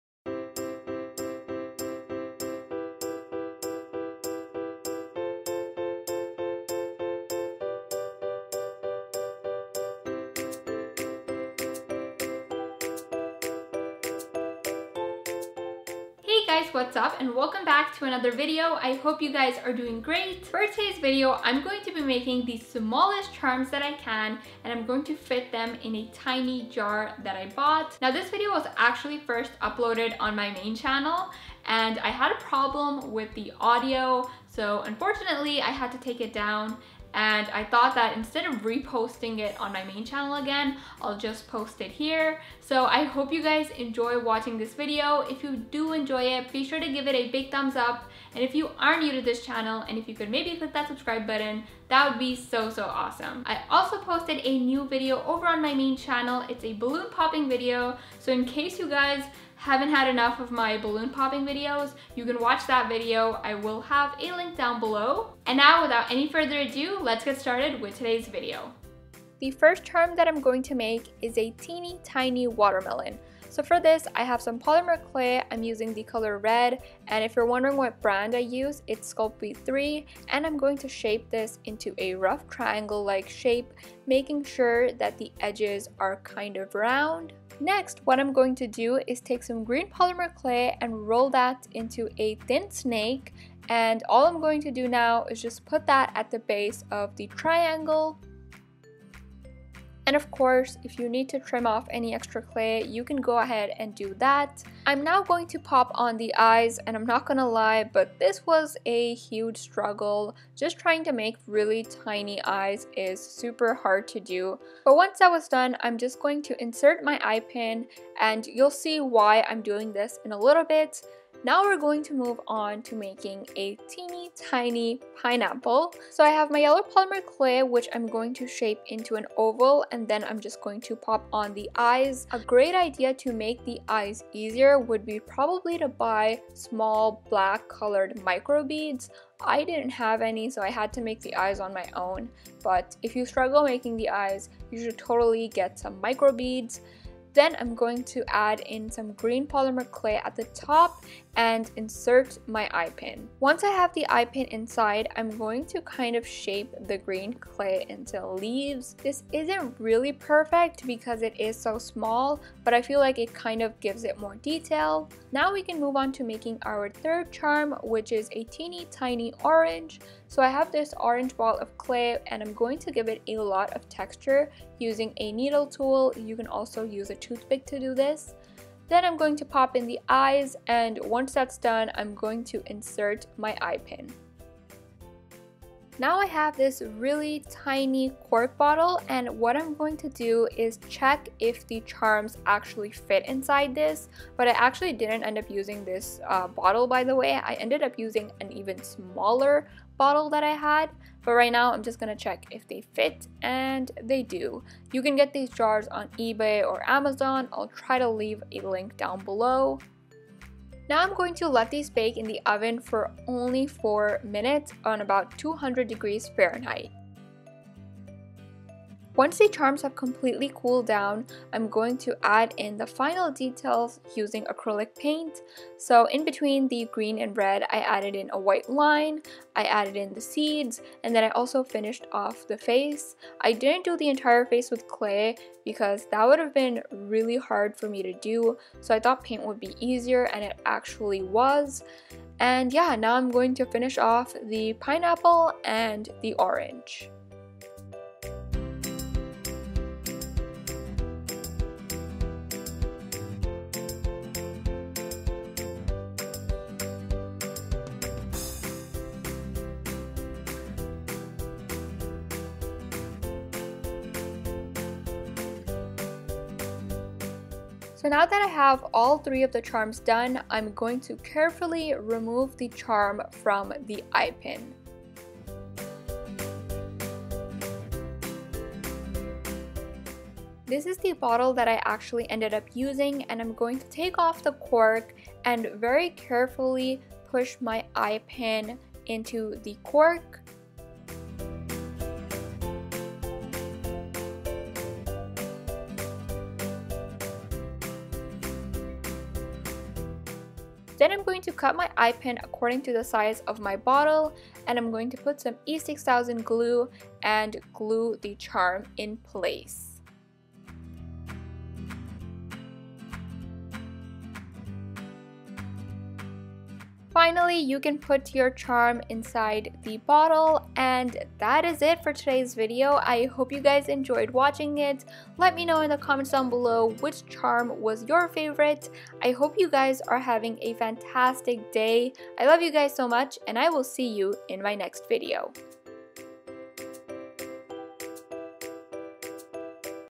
TRYING TO MAKE MINI CHARMS IN A BOTTLE!
Hey everyone!! I hope you all enjoy the video - Nim XO